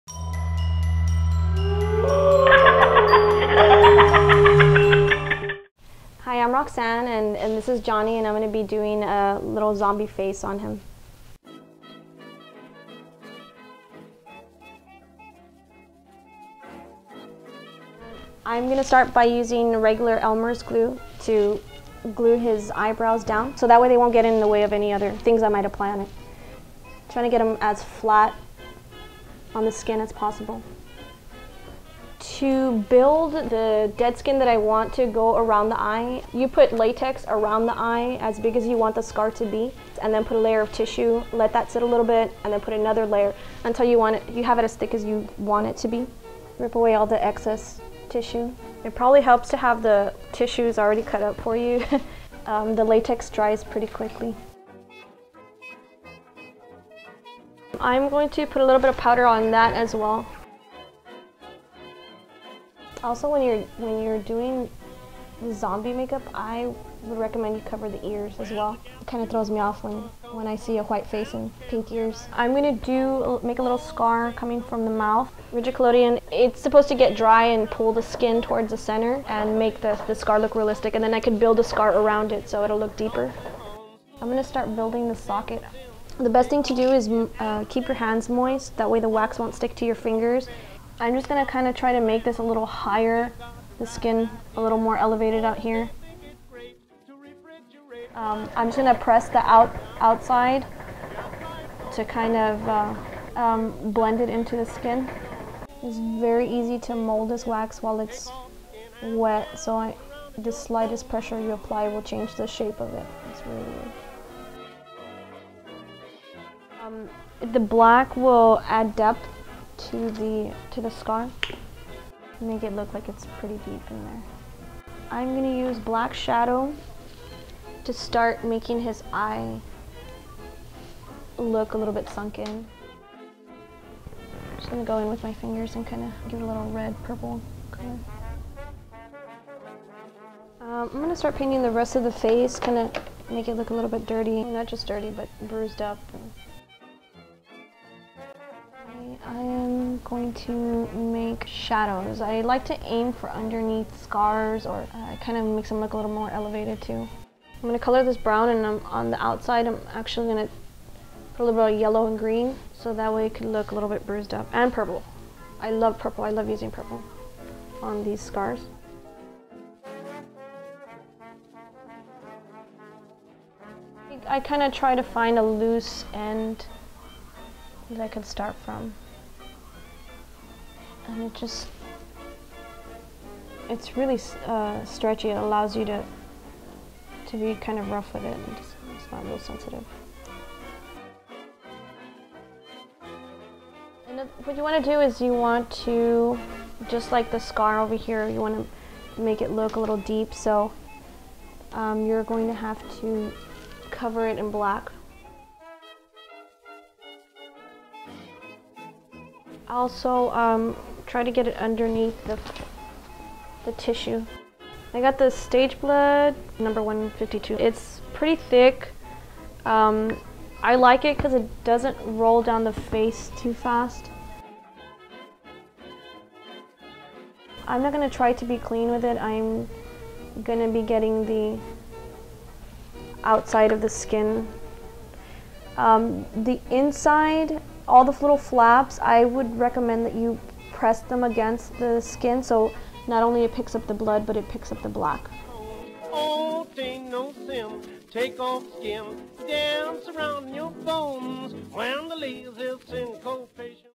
Zombie Make Up in 11 Steps
With zombie walks in season, Roxana Galvan shows us her version of the real walking dead.